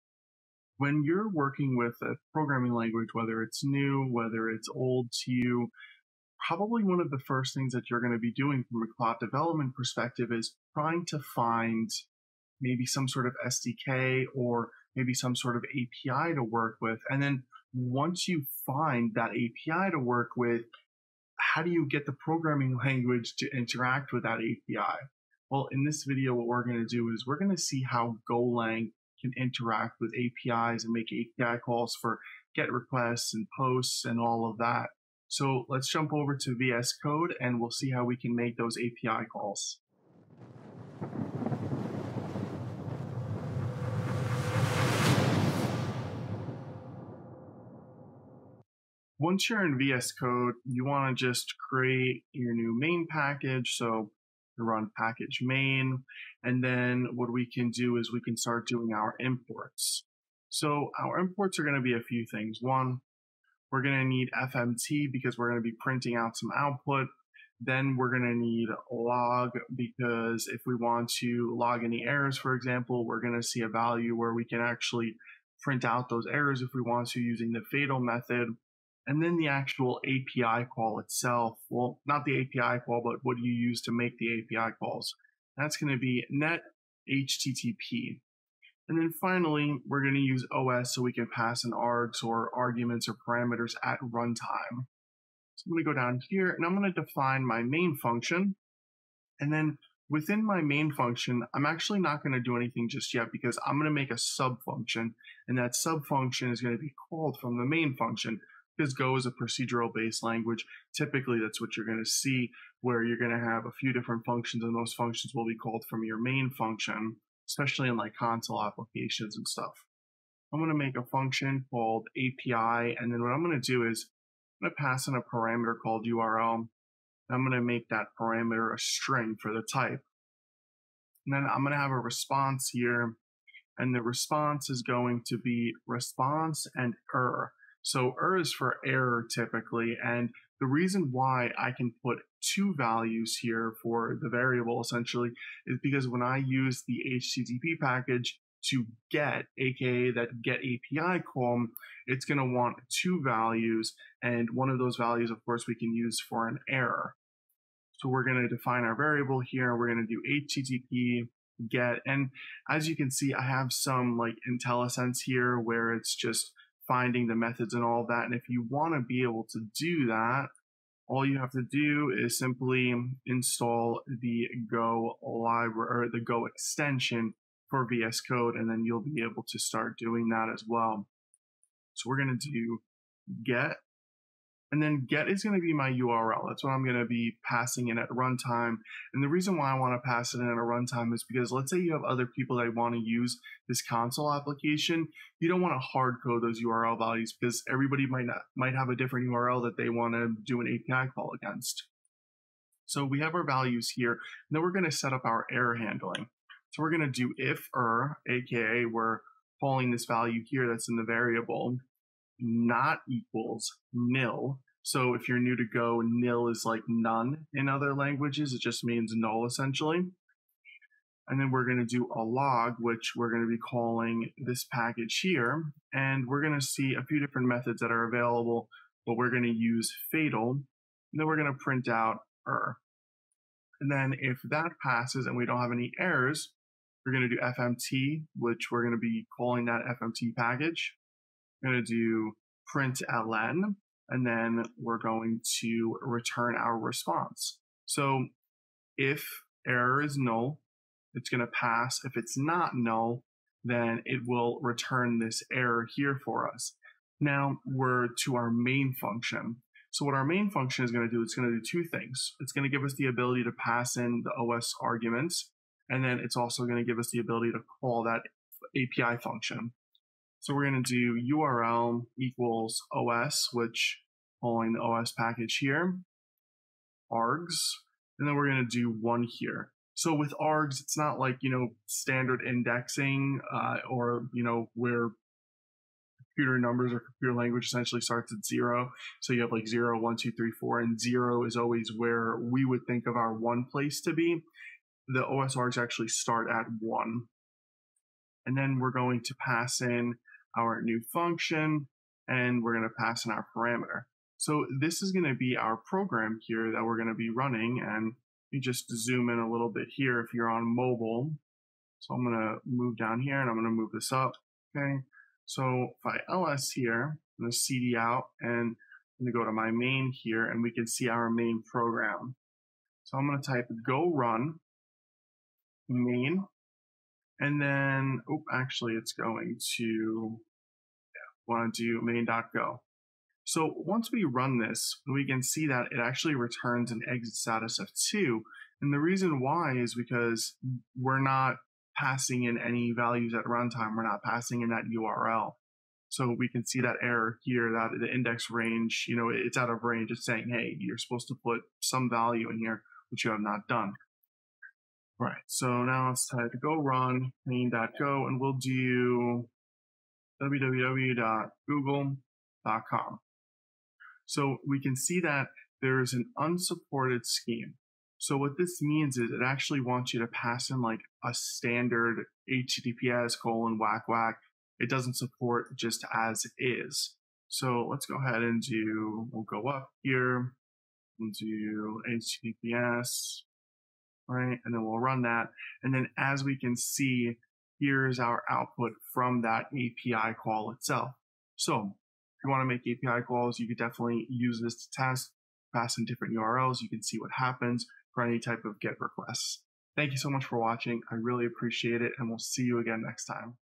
API Requests with Golang
When you're working in a programming language, whether you're using it for cloud development, automation, or tying in an application with a specific platform, 9 times out of 10 you'll need to work with an API.

But how do you actually work with the API? What packages do you need?

In this video you'll learn all about:
- Interacting with APIs in Go
- Using the net/http package in Go
- Passing in URLs at runtime to make API calls